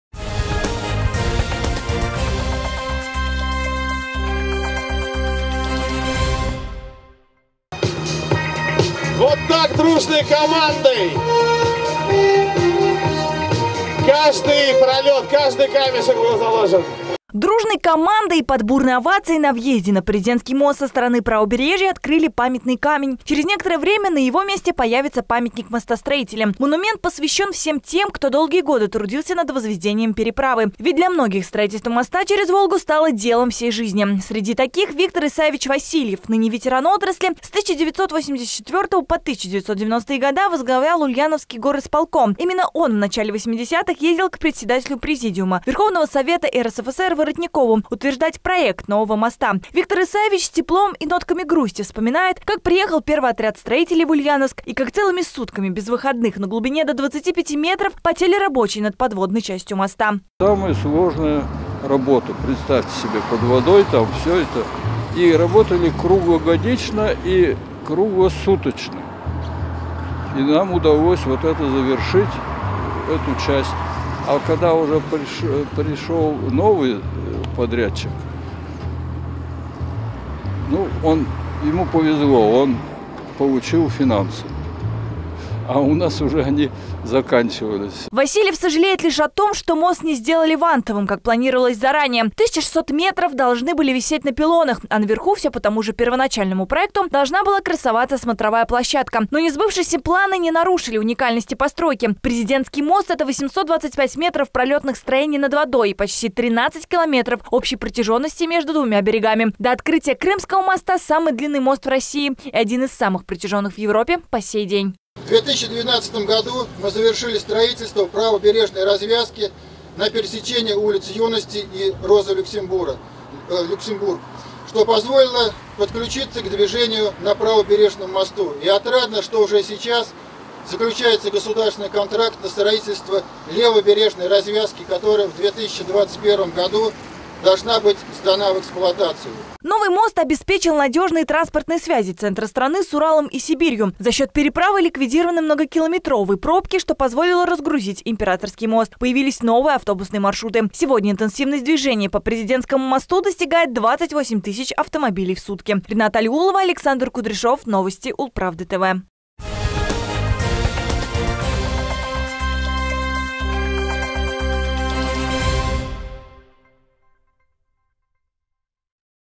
В благодарность и на память. В Ульяновске поставили монумент в честь мостостроителей
Местные жители привыкли до сих пор называть его новым, хотя с   начала движения по Президентскому мосту минуло 10 лет. Официальное открытие  переправы состоялось 24  ноября 2009 года.  Теперь память о тех, кто строил переправу, увековечили.